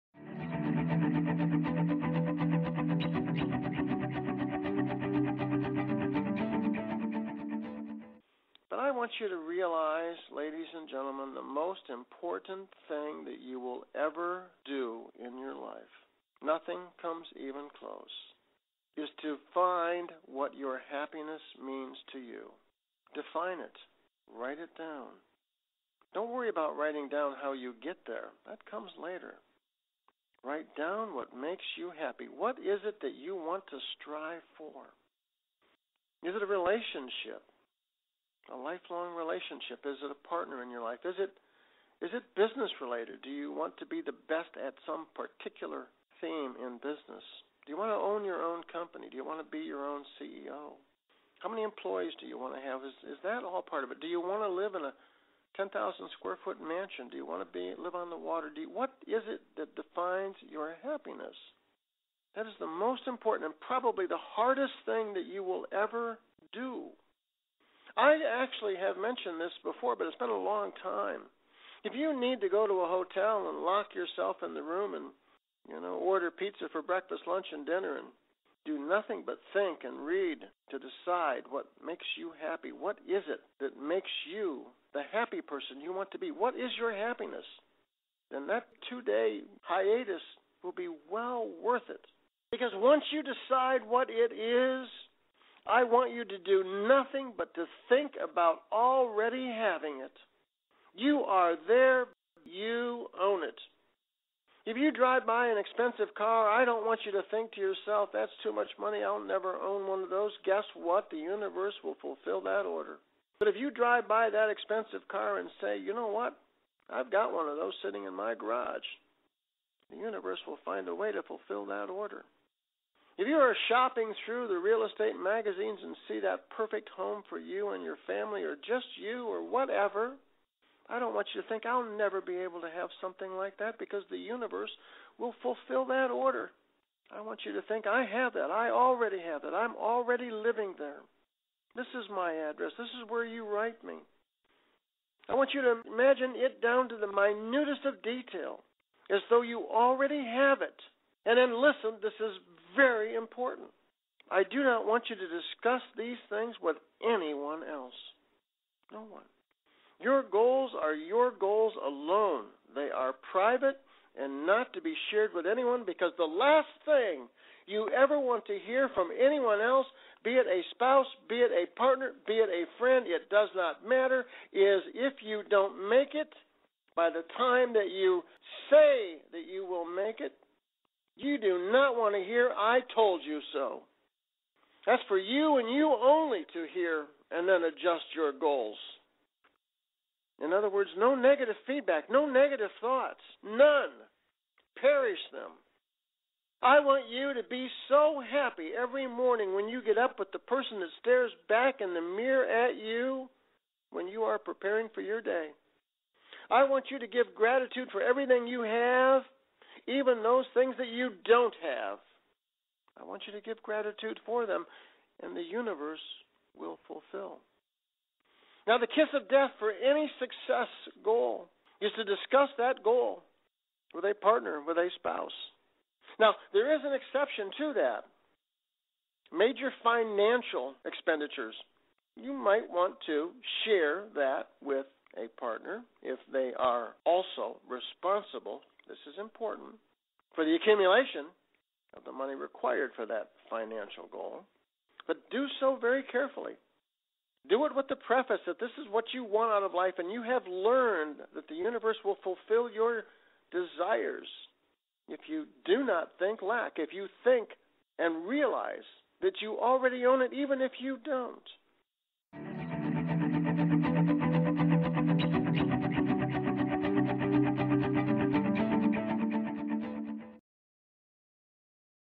Finding Your Happiness
"Finding Your Happiness" by Michael T Glaspie - (Mike G)

The most important thing your will ever do is to find
what your happiness means to you. Define it and write
it down - don't worry about getting there now. Are you
striving for a lifelong relationship, a partnership,
your own business? Do you want to own your own company
- and be the CEO, have employees? Do you want to live in a 10,000 square foot mansion, live near the water?
What is it that defines your happiness - probably the hardest thing you will ever do. Do you need to lock yourself in a hotel room and
deliberate on finding your happiness?
Once you decide what it is - do nothing but think that you already have it. You Own It. Want an expensive car - think that you already own it.
If you are shopping through RealEstate magazines and
see the perfect home - believe that you already have it
and the Universe will arrange for it to happen. Just believe that you already live there.DO NOT discuss your goals with anyone else - they are your private goals and not to be shared with anyone else, because if you don't make them you don't want to hear "I Told You So". Adjust those goals and avoid all negative feednback. Be happy with yourself every day. Be gratefull for eveything you have and even those things you don't have because the Universe will fullfil them.The kiss of death to reaching your success goals is to
discuss them with a spouse or partner except for major financial expen ditures if they are partially responsible for accumulating those funds. But be careful - these are your goals and the things you want and you have learned that the Uiverse will fullfil your
desires when you THINK that you already have them -
even if you don't.
The ICANetwork - Mobile Conversions - Local SEO - SmartphMike G offersone Apps.